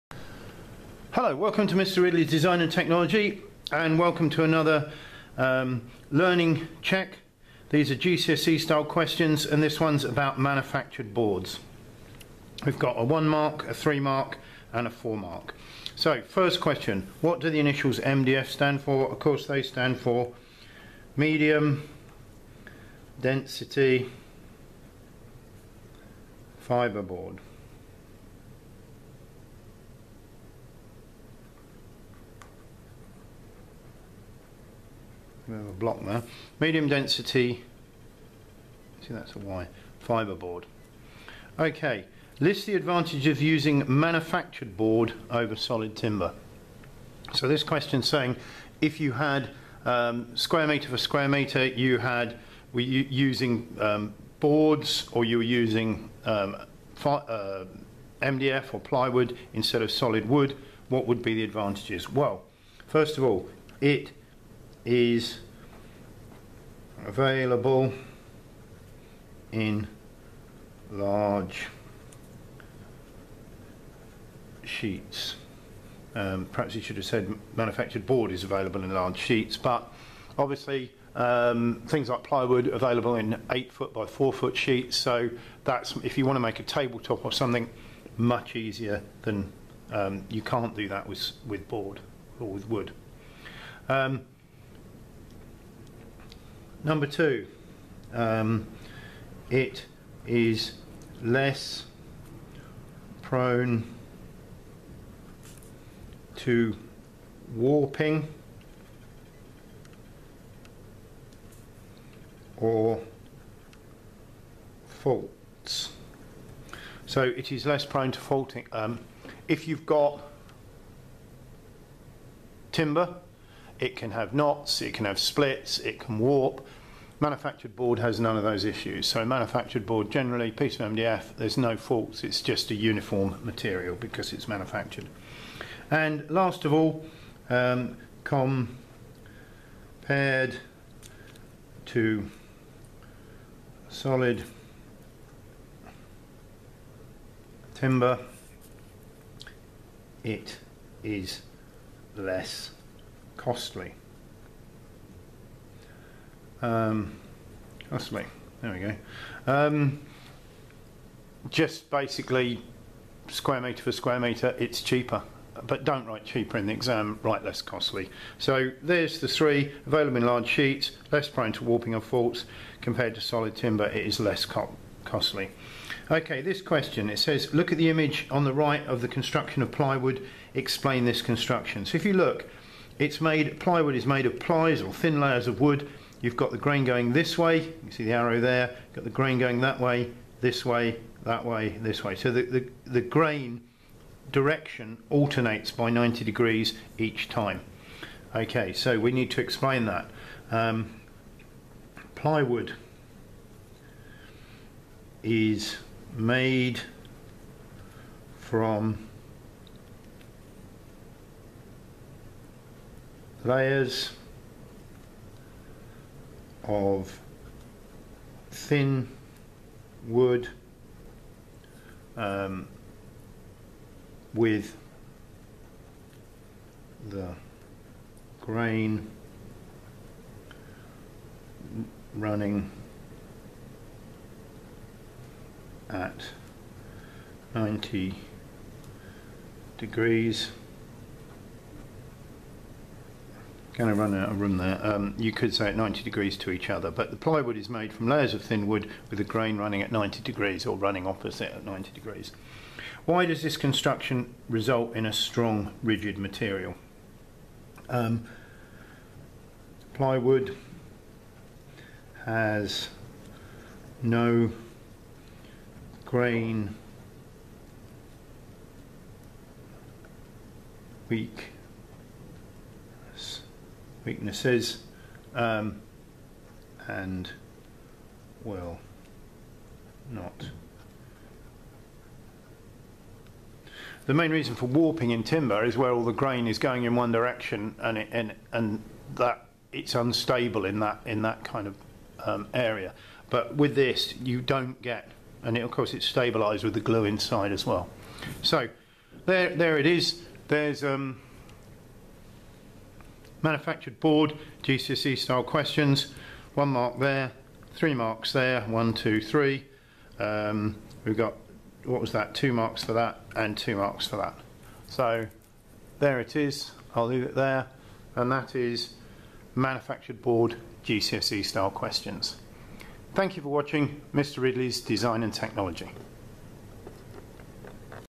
Learning Check Manufactured Boards GCSE Style Questions
This is a quick learning check covering Manufactured boards for GCSE D&T. these are 3 GCSE style questions.  This can be used as part of a lesson or just watch as part of your D&T revision. This sheet is available together with the main worksheet, from my TES shop @.